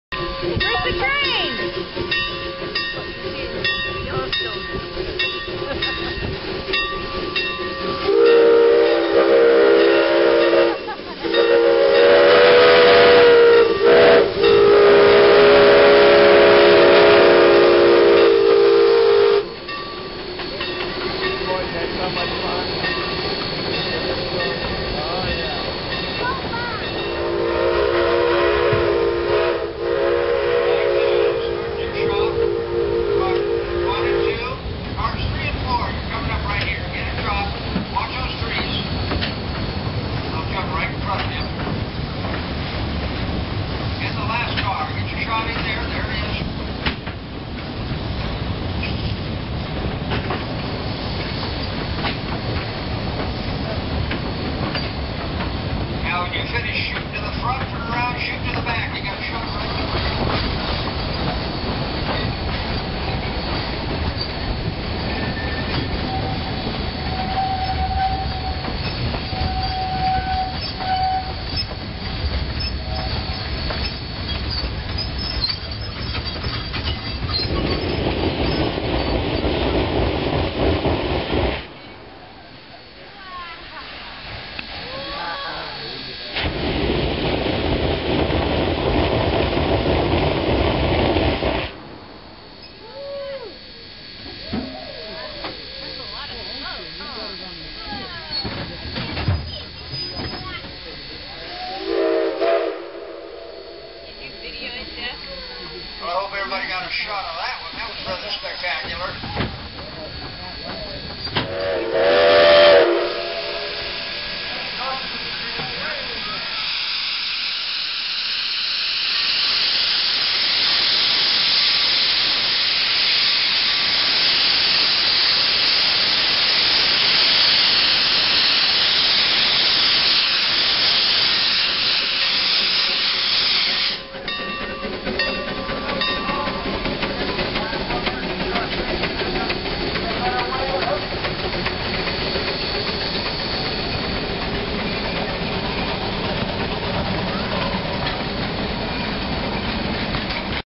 "Sonora" In The Mist - A cold day at Roaring Camp
A brief video montage of the wonderful Shay locomotive "Sonora" at Roaring Camp on a very cold day. I wish I had remembered to bring the video camera, all of these were taken on my digital still camera. Steam trains are always at their best in the cold though.